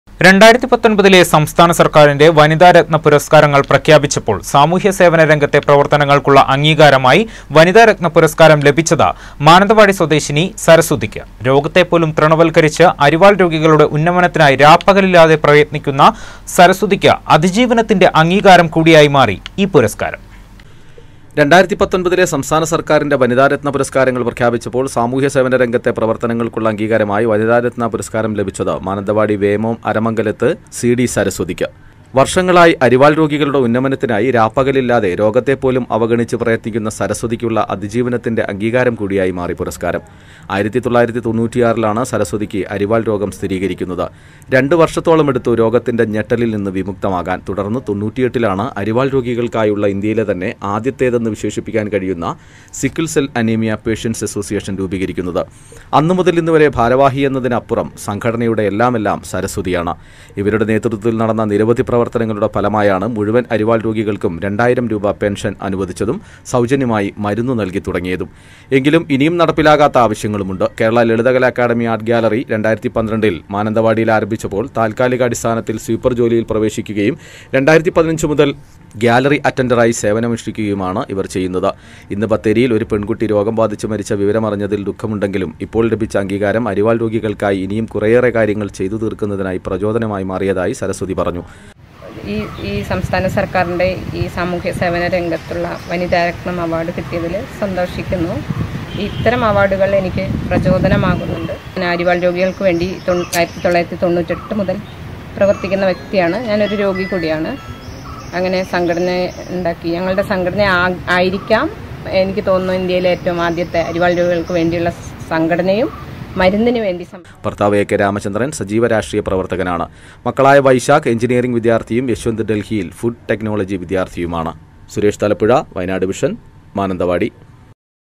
സരസ്വതിക്ക് അതിജീവനത്തിന്റെ അംഗീകാരം.
വനിതാ രത്‌ന പുരസ്‌ക്കാരം ലഭിച്ചത് മാനന്തവാടി സ്വദേശി സരസ്വതിക്ക്.